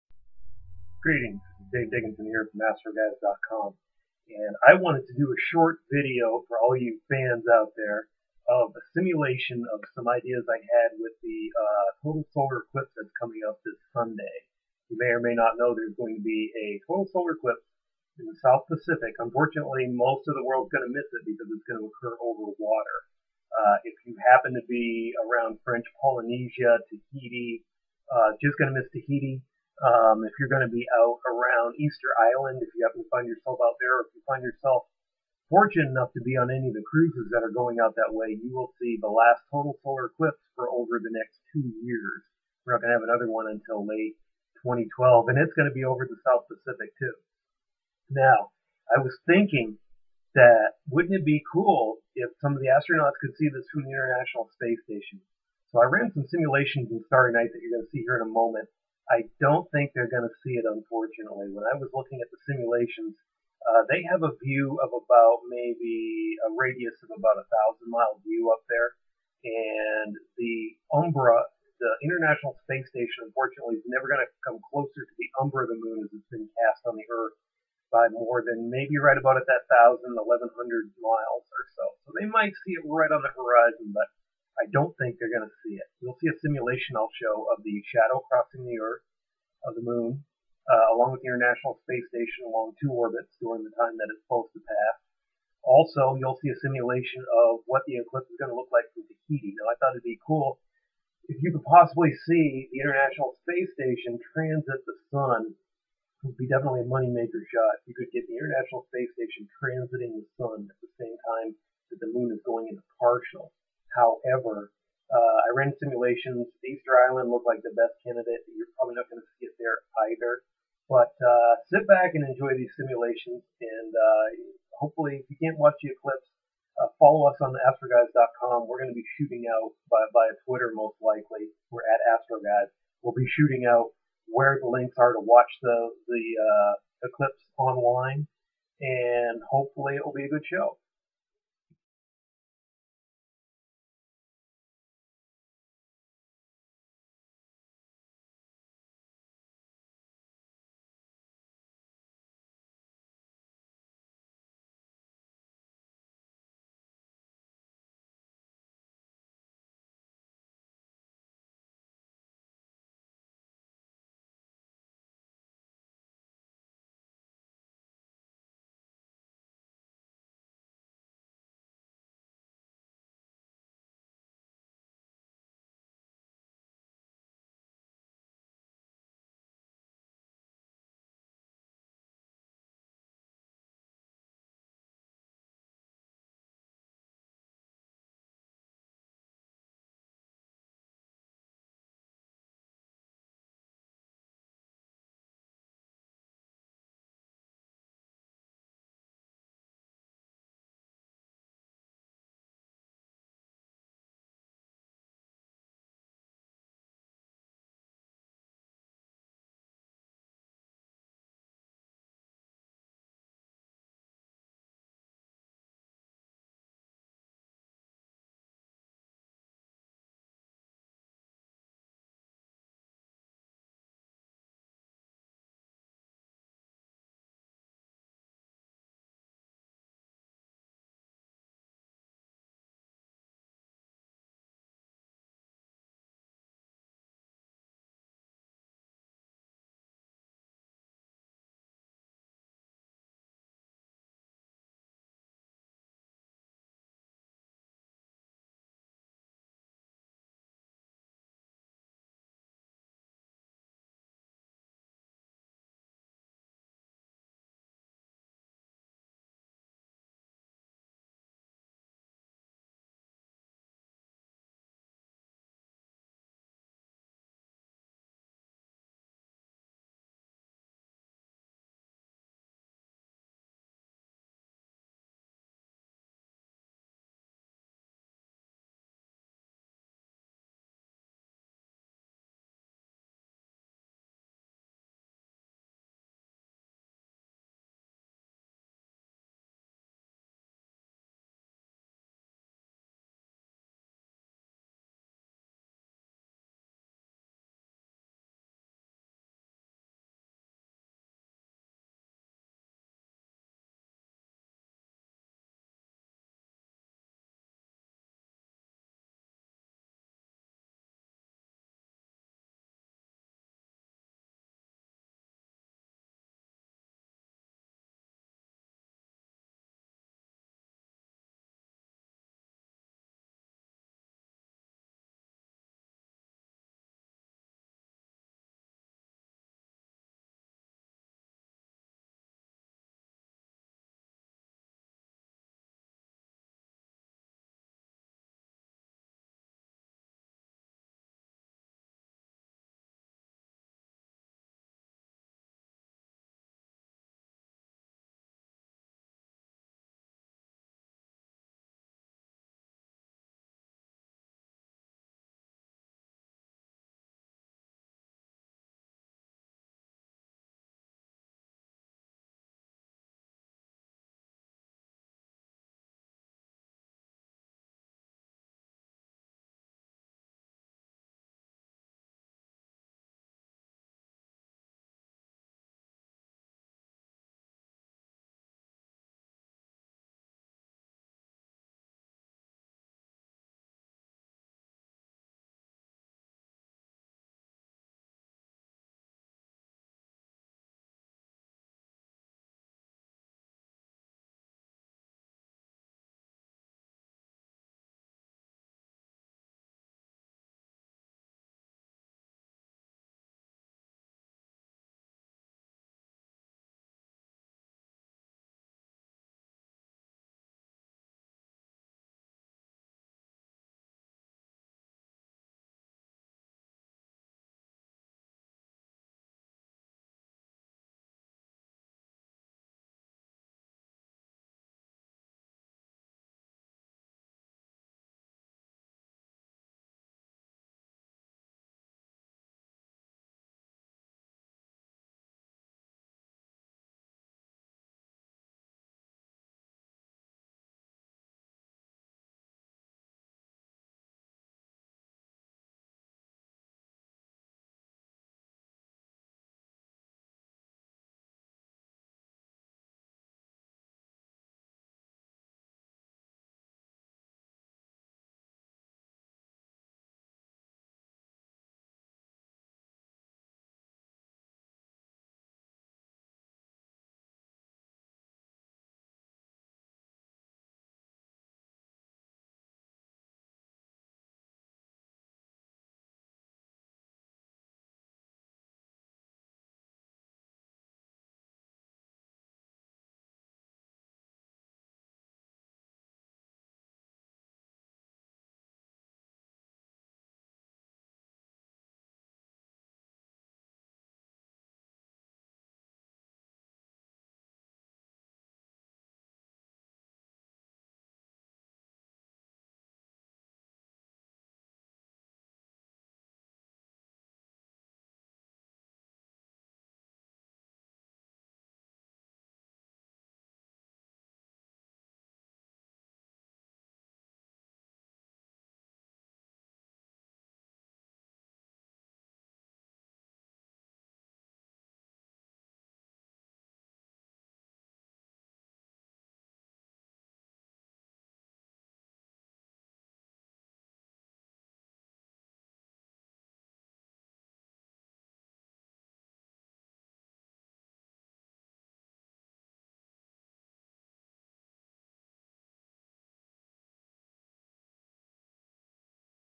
A South Pacific Total Solar Eclipse.
A simulation of the upcoming Total Solar Eclipse as seen from Easter Island, Including simulations of the ISS orbit during the eclipse an the view from the ISS.